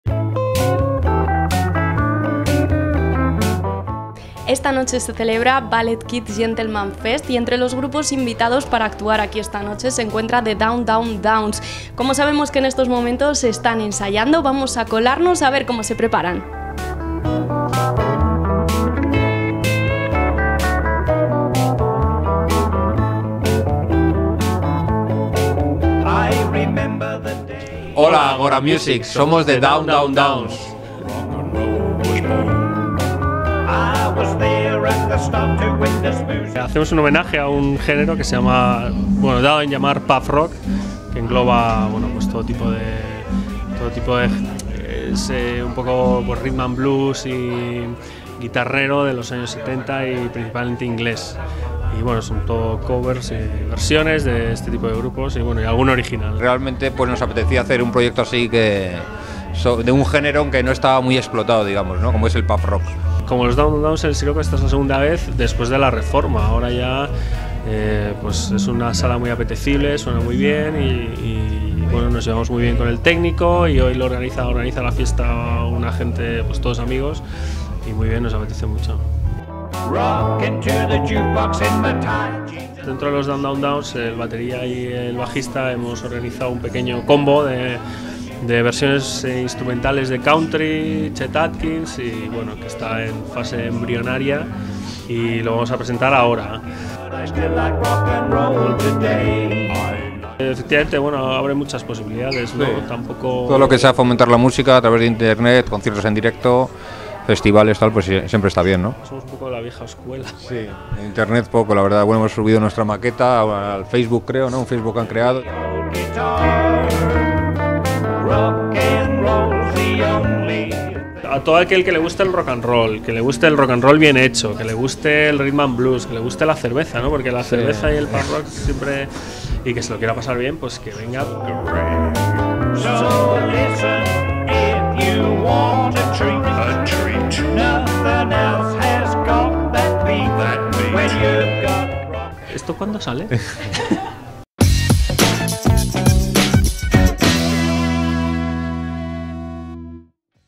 Entrevista a The Down Down Downs en la sala Siroco
El pasado viernes estuvimos en la sala Siroco, en el ensayo del concierto de un mítico grupo madrileño, The Down Down Downs.
¡No os perdáis la entrevista que les hicimos!